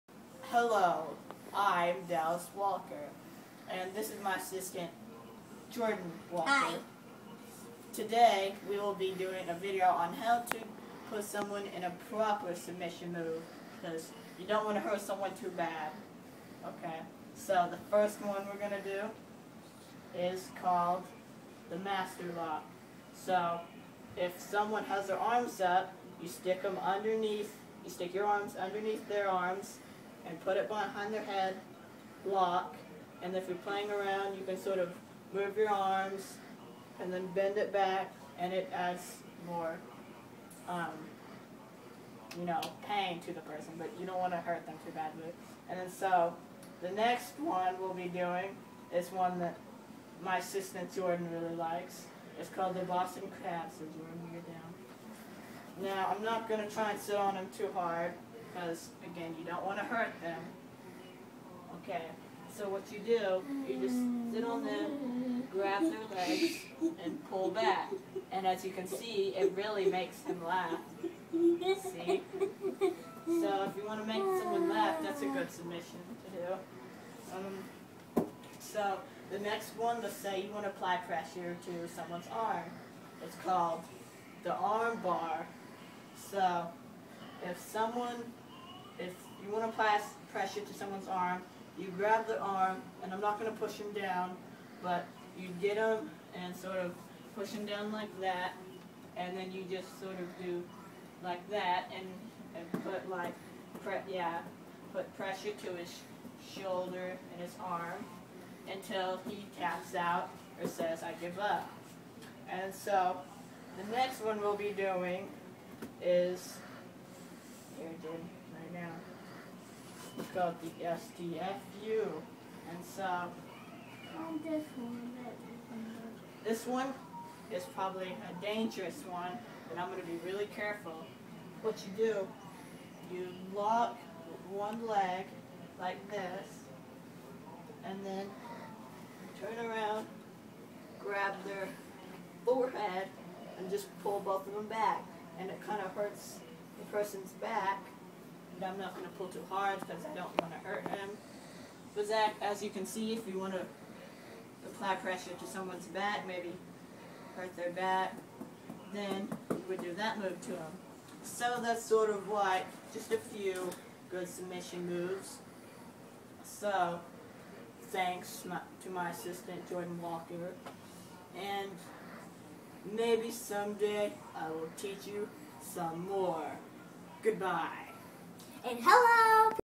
How to do some some submission holds
Dallas doing his "how to" video for his GT class